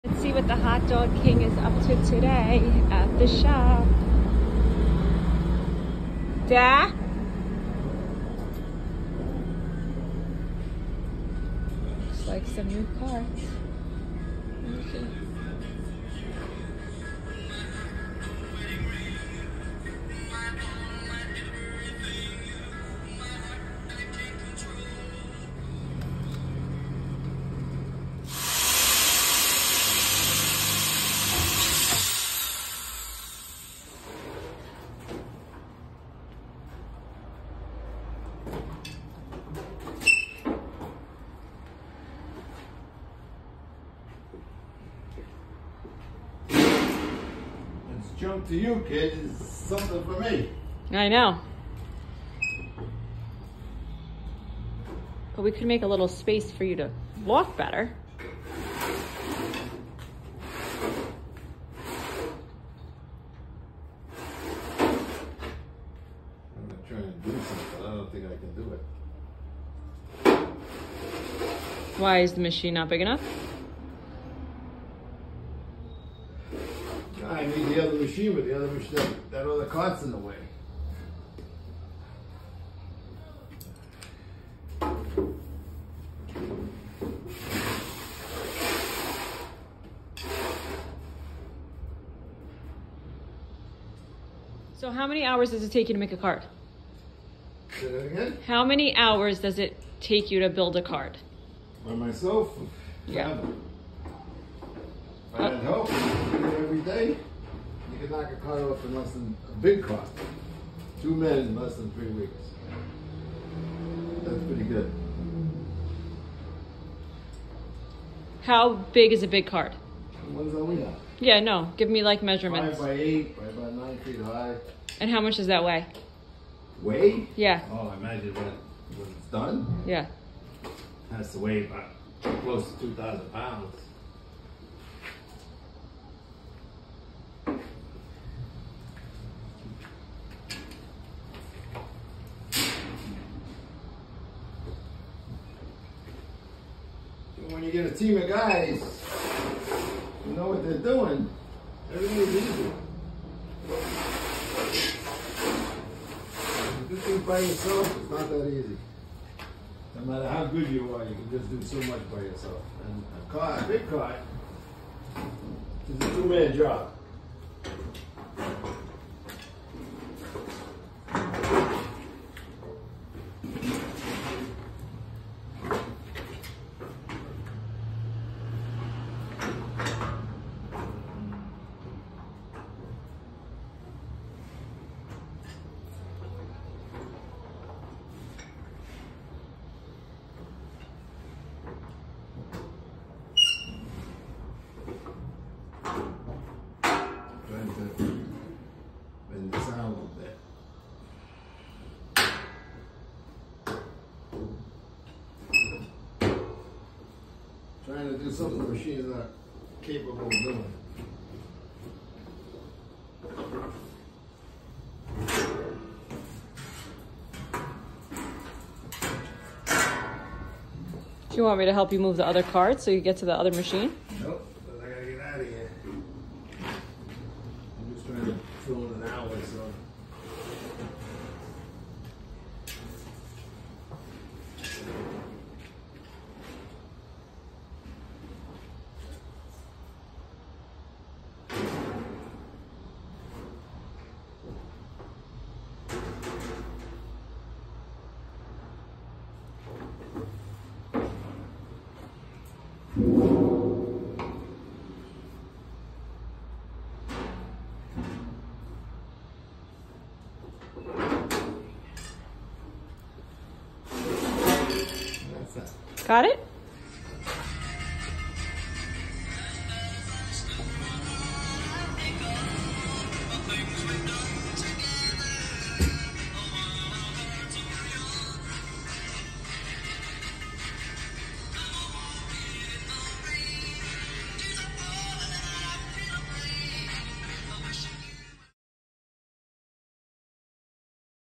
Behind the scenes with Dan
Did you know Dan makes all of his carts?
He’s been building carts for the last 40 years. Let us know if you want to see more videos like this.
And if you want to read about Dan’s life, pick up a copy of his book. The Hot Dog King: From Rags to Riches to less than Rags.